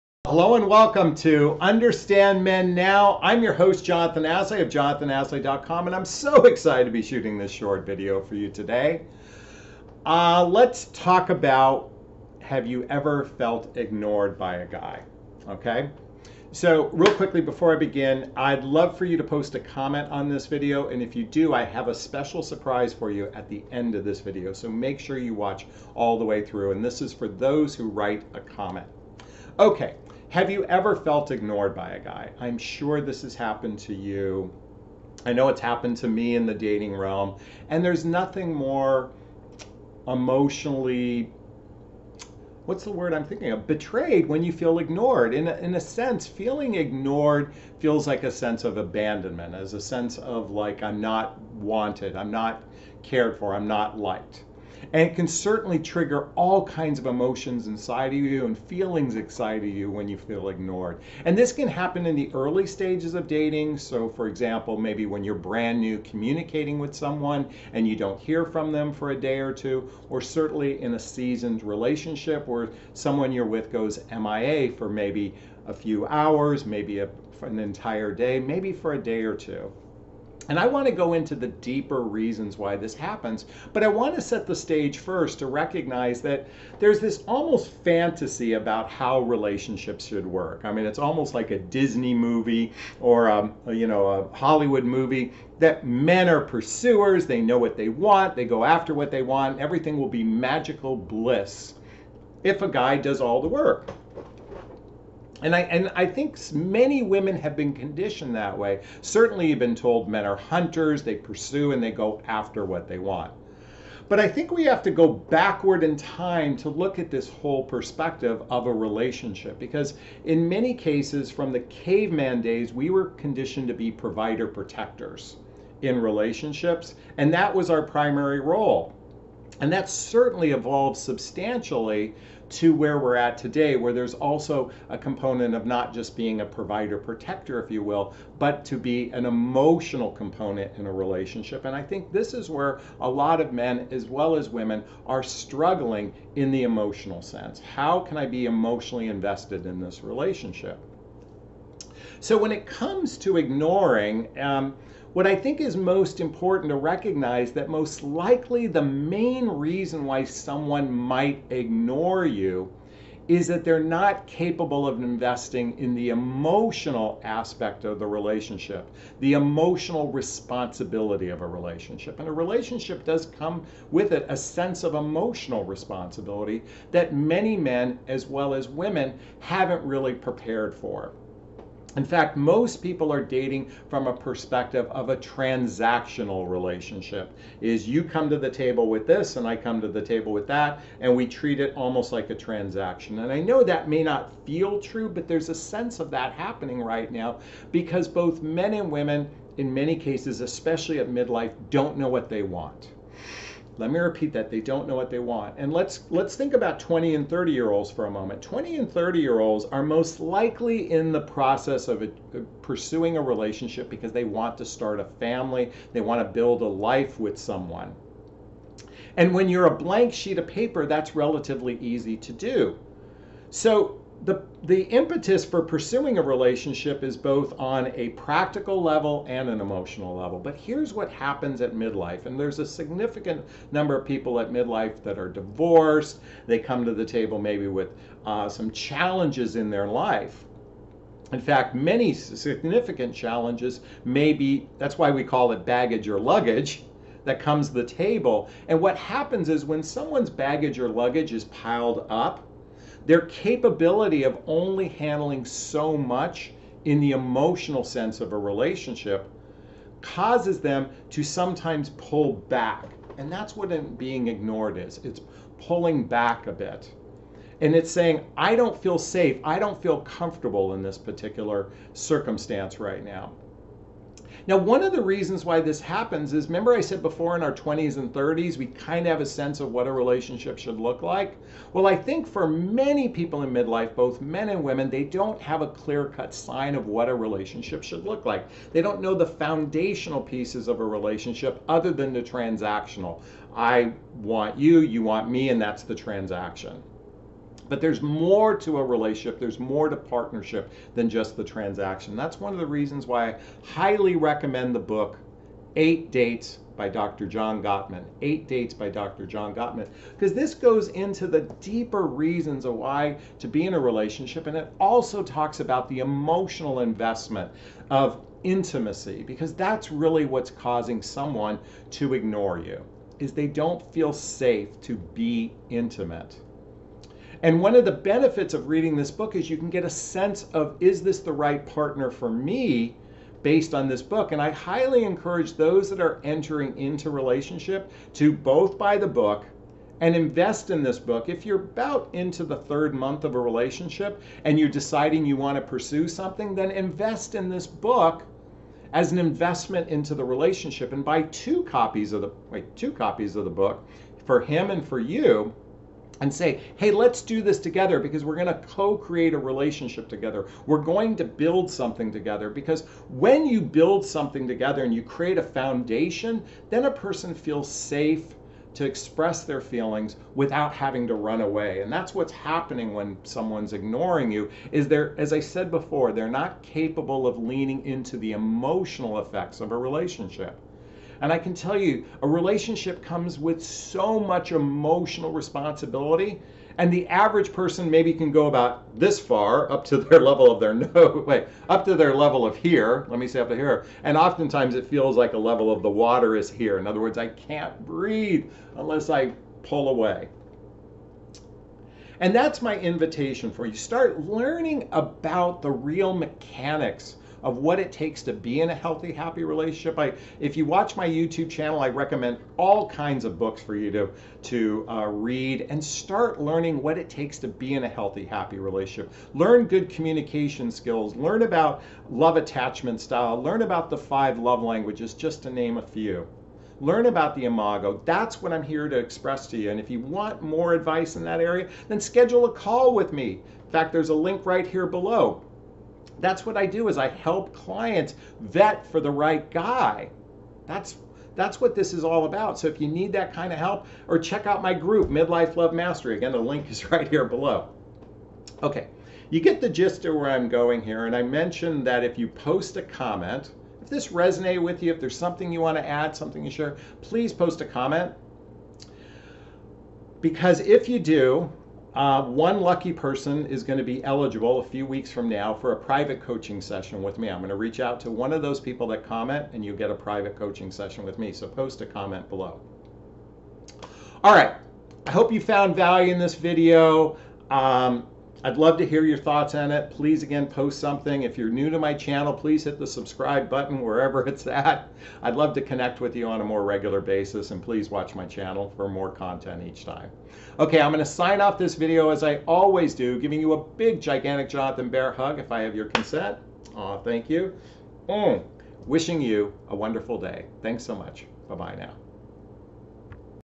When A Man Ignores You : The SHOCKING Reason
When A Man Ignores You, Here's The SHOCKING Reason

It's rather unsettling to care for someone and then feel ignored or discounted, am I right?

If you're like me, it almost feels like a betrayal of connection or worse, a feeling that something is wrong with us. Whether it's our first couple interactions with someone or a person we are in a relationship with, even a moment of MIA can feel like a lifetime. On an emotional level, being ignored can create inner havoc and make us question our own worth.

Let me say this clearly, there is nothing wrong with us and there is often a deeper reason why this happens, especially with men.

Men often ignore (women) for a reason that is foreign to women. Most women have a skill set that many men lacking, which is why men's actions can seem very unnerving.

In today's video, we are going to explore an area of inner strength men could sorely use help with when it comes to dating, mating & relating.

Let's talk about... When A Man Ignores You, Here's The SHOCKING Reason